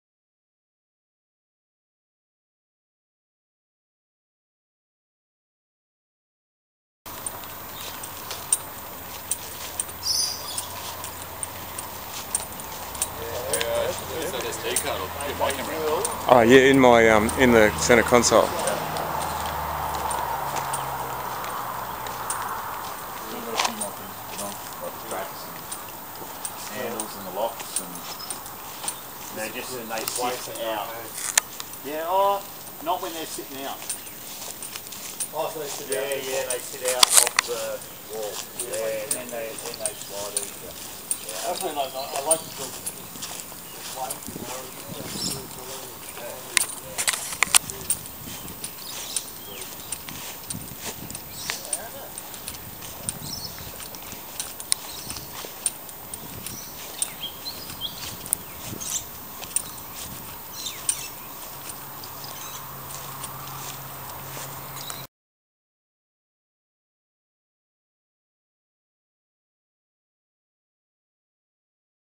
Grand Palace Movie
"The Grand Palace" cabin kit part 1. "The Grand Palace" is a 60m2 2 bedroom cabin kit with a huge 1.8 metre covered verandah in solid 70mm Timber with 28 thick timber floor and tongue and grove timber ceiling.
Granny Flat, Cabin Kits.